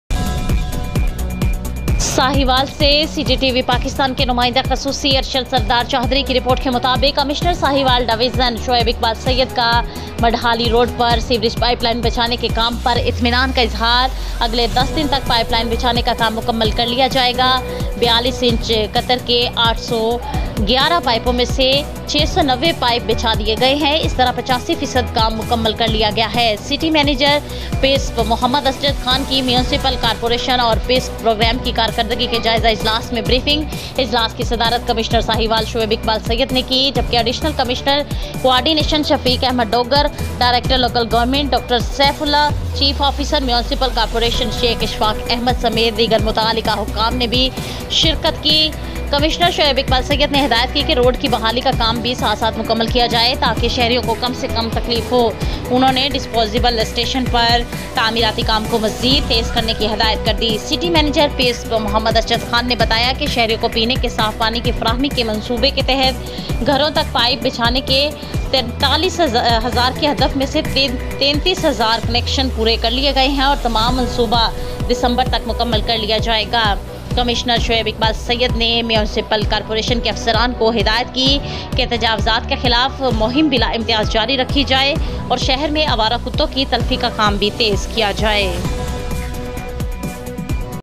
Commissioner Sahiwal Division performance review meeting of Municipal Corporation and PSP program
کمشنر ساہیوال ڈویژن شعیب اقبال سید کا مڈھالی روڈ پر
 سیوریج پائپ بچھانے کے کام پر اطمینان کا اظہار، اگلے دس دن تک پائپ لائن بچھانے کا کام مکمل کر لیا جائے گا، 42انچ قطر کے811پائپوں میں سے690بچھا دیے گئے ہیں اس طرح 85فی صد کام مکمل کر لیا گیا ہے، سٹی مینجر پسپ محمد اسجد خان کی میونسپل کارپوریشن اور پسپ پروگرام کی کارکردگی کے جائزہ اجلاس میں بریفنگ۔ اجلاس کی صدارت کمشنر ساہیوال شعیب اقبال سید نے کی جبکہ  ایڈیشنل کمشنر کوارڈینیشن شفیق احمد ڈوگر،  ڈائریکٹر لوکل گورنمنٹ ڈاکٹر سیف اللہ،  چیف آفیسر میونسپل کارپوریشن شیخ اشفاق احمد سمیت دیگر متعلقہ احکام نے بھی شرکت کی۔کمشنر شعیب اقبال سید نے ہدایت کی کہ سڑک کی بحالی کا کام بھی ساتھ ساتھ مکمل کیا جائے تاکہ شہریوں کو کم سے کم تکلیف ہو۔ انہوں نے ڈسپوزل سٹیشن پر تعمیراتی کام کو مزید تیز کرنے کی بھی ہدایت کی۔ سٹی مینجر پسپ محمد اسجد خان نے بتایا کہ شہریوں کو پینے کے صاف پانی کی فراہمی کے منصوبے کے تحت گھروں تک پائپ پہنچانے کے 43 ہزار کے ہدف میں سے 33ہزار کنکشن پورے کر لئے گئے ہیں اور تمام منصوبہ دسمبر تک مکمل کر لیا جائے گا۔ کمشنر شعیب اقبال سید نے میونسپل کارپوریشن کے افسران کو ہدایت کی کہ تجاوزات کے خلاف مہم بلا امتیاز جاری رکھی جائے اور شہر میں آوارہ کتوں کی تلفی کا کام بھی تیزکیا جائے۔